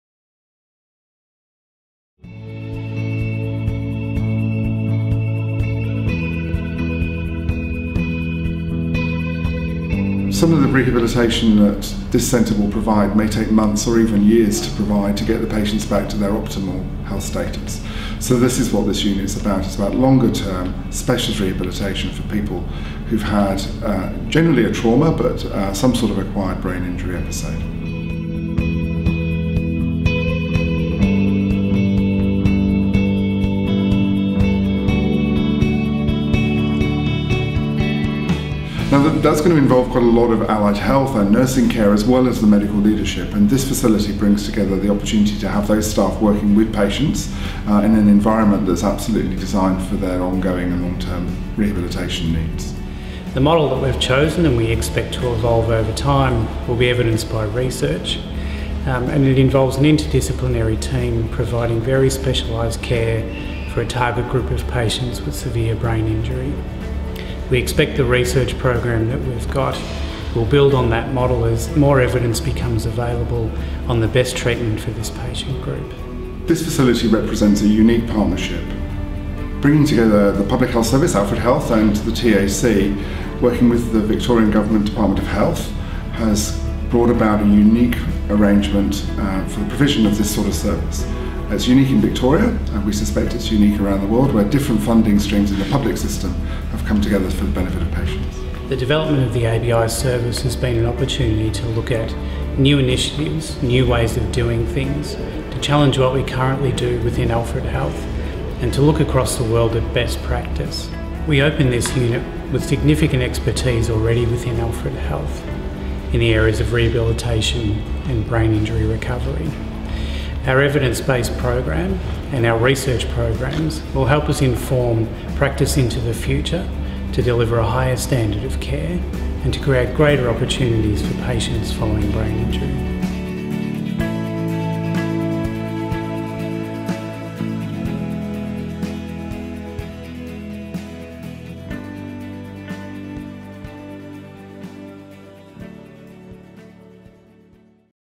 Introducing our Acquired Brain Injury Rehabilitation Centre
Victoria’s first purpose-built centre of excellence for brain injury rehabilitation opened at Caulfield Hospital in September 2014.

The $36m Acquired Brain Injury (ABI) Rehabilitation Centre expands an already developed sector of services for people living with acquired brain injury – and heralds a new era of specialist care for patients with acquired brain injury.

The centre features a 42-bed inpatient rehabilitation service, four-bed transitional living service, and an ABI community rehabilitation service.